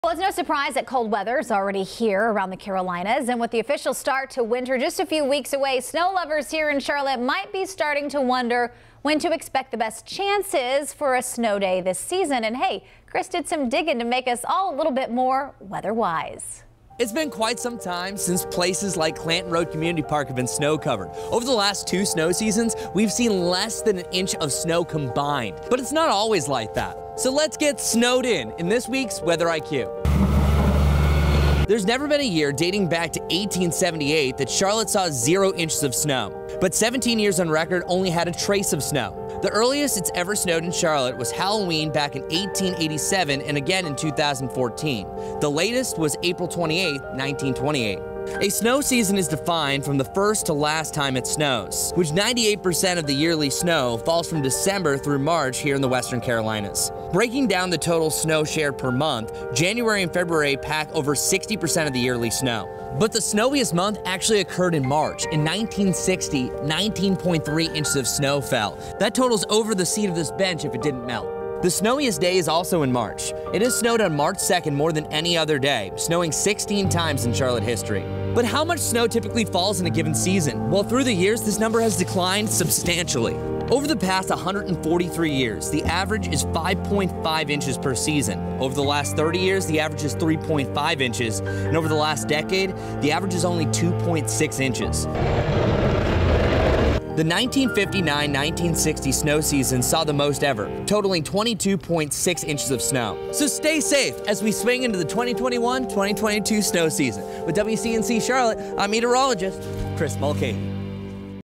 This is when to expect snow in Charlotte
Recent snow and snow statistics
Over the last two years, Charlotte has seen less than an inch of snow total. Charlotte has received only 0.6" of snow total across the 2019-20 and 2020-21 snow seasons.

There has never been a year dating back to 1878 that Charlotte saw zero snow. But 17 years on record only had a trace of snow.

The earliest it has ever snowed in Charlotte was Halloween back in 1887 and 2014. The latest was April 28, 1928. Both the earliest and latest snowfalls were only a trace of snow.

The average snow season
But how much snow typically falls per snow season? Over the years, this number has declined substantially.

Over the past 143 years, the average is 5.5" per season. Over the last 30 years, the average is 3.5" and over the last decade, the average is only 2.6" per season.

Also, La Niña years typically see less snow than other snow seasons and the period between the ENSO cycles (La Niña and El Niño) has averaged the most snowfall.

More Charlotte snow stats
A snow season is defined from the first to last time it snows in a season in which 98% of the yearly snowfall comes from December through March in the western portion of the Carolinas.

Breaking down the total snow share by month, January and February pack over 60% of the yearly snow.

But the snowiest month on record actually occurred in March back in 1960 when 19.3" of snow fell.

This record month was a part of the snowiest season. The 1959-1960 snow season saw the most ever: Totaling 22.6" of snow.

The snowiest day is also in March. It has snowed on March 2 more than any other day, snowing 16 times in Charlotte history.

The most snowfall to fall in one day was 14 inches set back on Feb. 15, 1902. This was all a part of the biggest Charlotte snowstorm in history that dumped over 17 inches total. (17.4" to be precise)